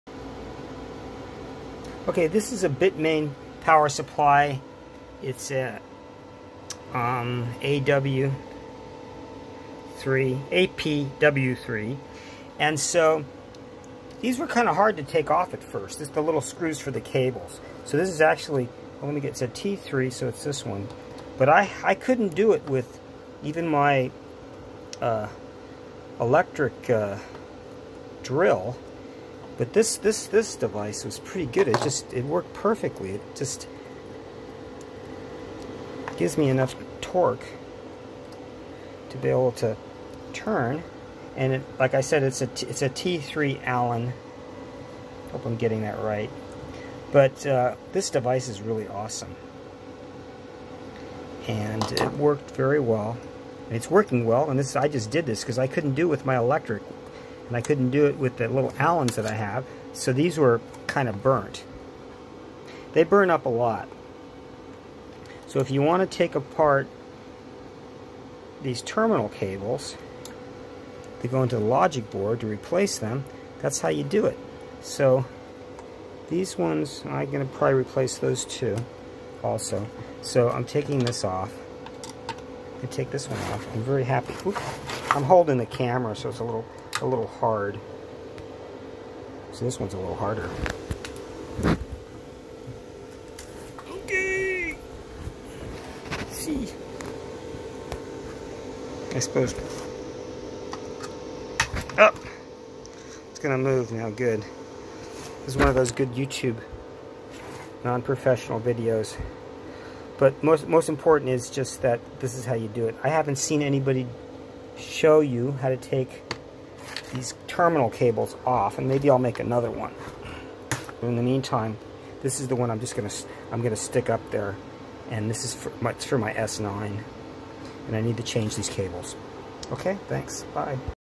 BITMAIN S9 APW3 Power Supply Terminal Cable Removal.
BitMain   APW3 Power Supply Terminal Cable Removal.
Use a  T3  Allen wrench for removal of the cables.